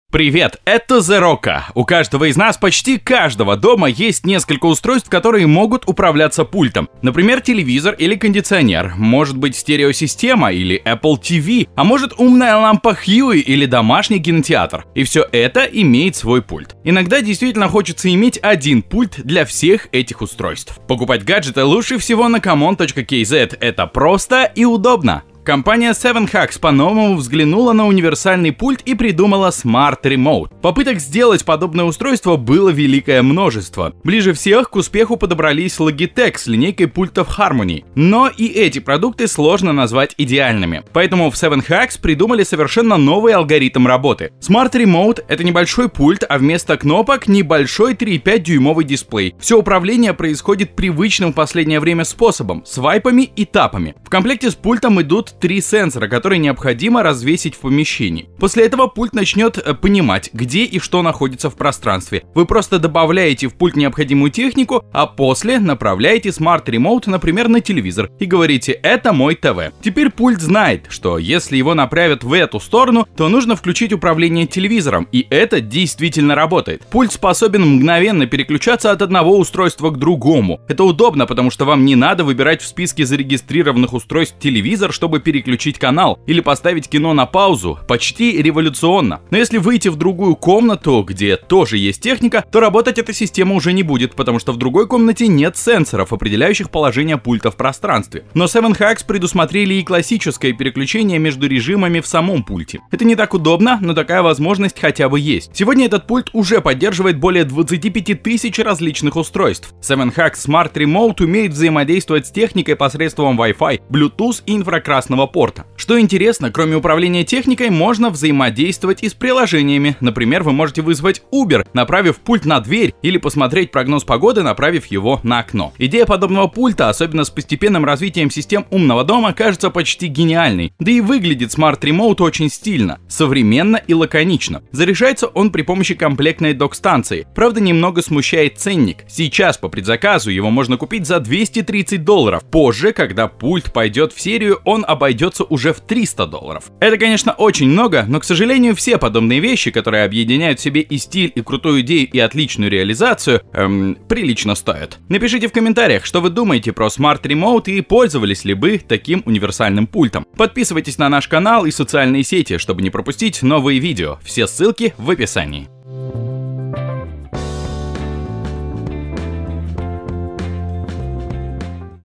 Умный пульт Sevenhugs Smart Remote
Один из лучших гаджетов CES 2017 - Sevenhugs Smart Remote. Этот универсальный пульт может управлять абсолютно всей техникой в доме, более того, вы можете вызвать Uber с его помощью или узнать погоду.

И кстати, ставь Like и мы будем знать, какие обзоры и сюжеты тебе нравятся больше. Впереди много очень крутых обзоров и новостей, не пропусти!